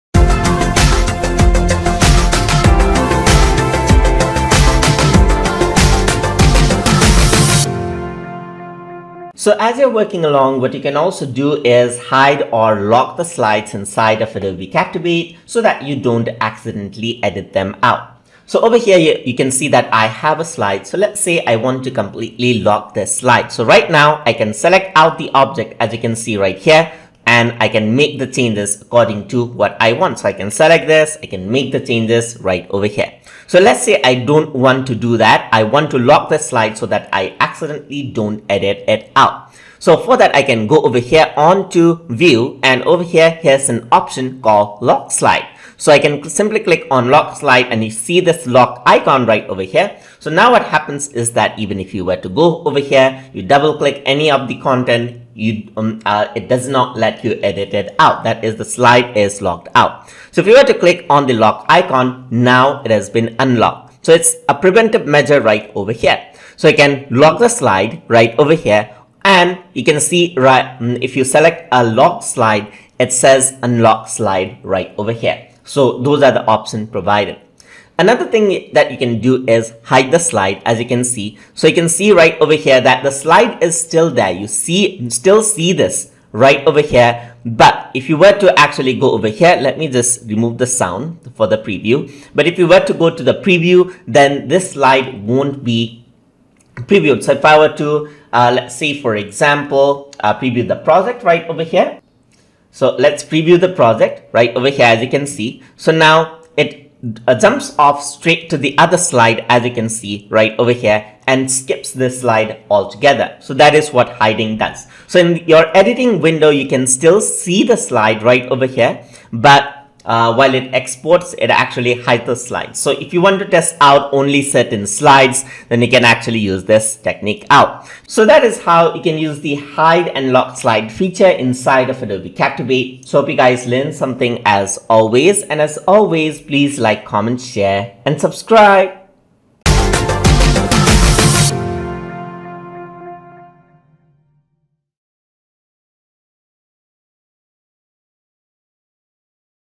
Adobe Captivate Tutorial - Lesson 76 - Hide and Lock Slides
In this tutorial, we will be discussing about Hide and Lock Slides in Adobe Captivate